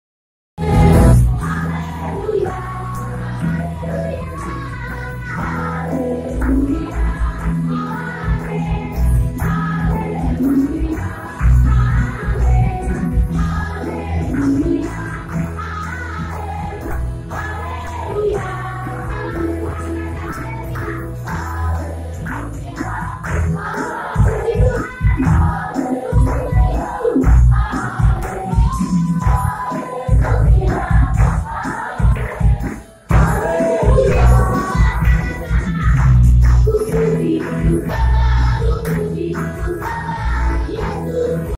IBADAH UMUM JAM 06 SORE DI GPDI IMANUEL NUSA DUA BALI
part 2
Ibadah 12February 2023,Setiap Minggu jam 6sore di GEREJA GPDI IMANUEL NUSA DUA BALI

Yang sedang liburan di Bali dan sangat rindu beribadah,bisa ibadah sore bersama keluarga.kerajaan Allah memuji dan menyembah Allah bersama,
pintu gerbang masuk sangat terbuka🤗